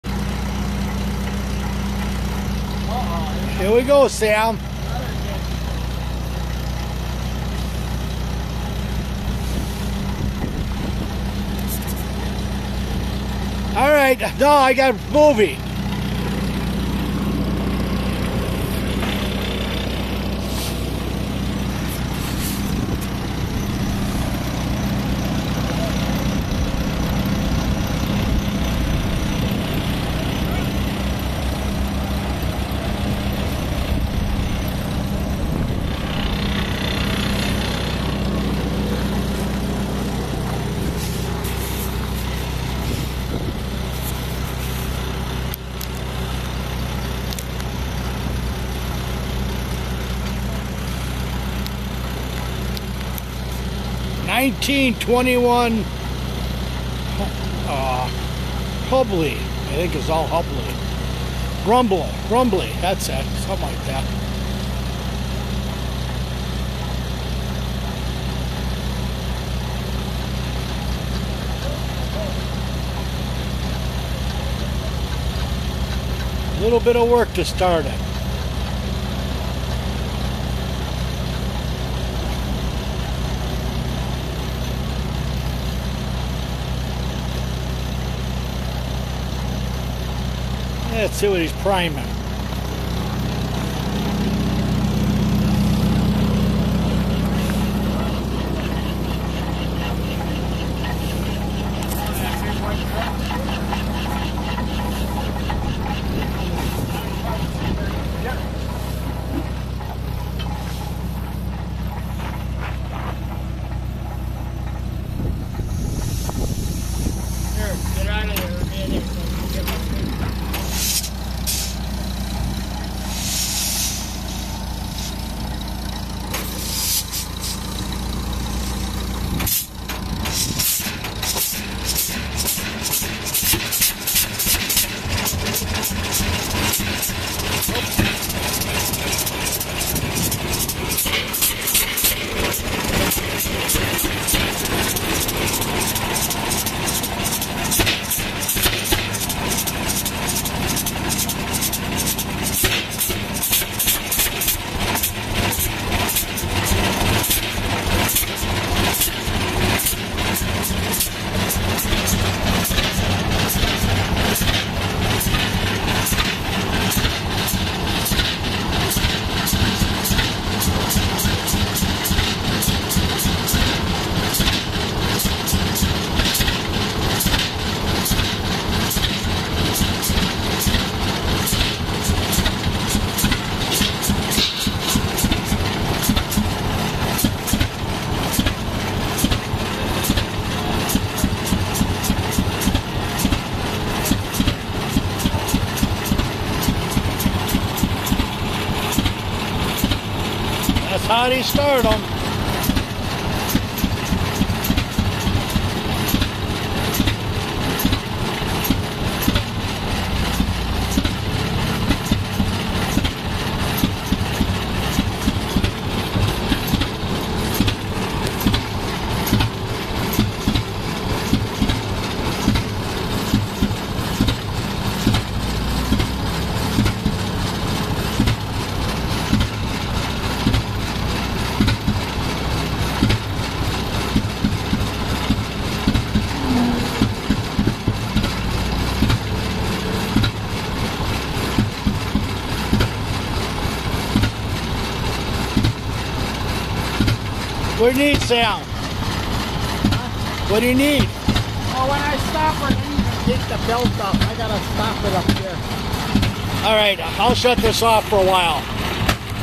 Rumely 1921 oil pull Starting at home after show
This 1921 Rumely  starting with the help of a farmall  and heading to the barn for storage after the Sussex WI show 2010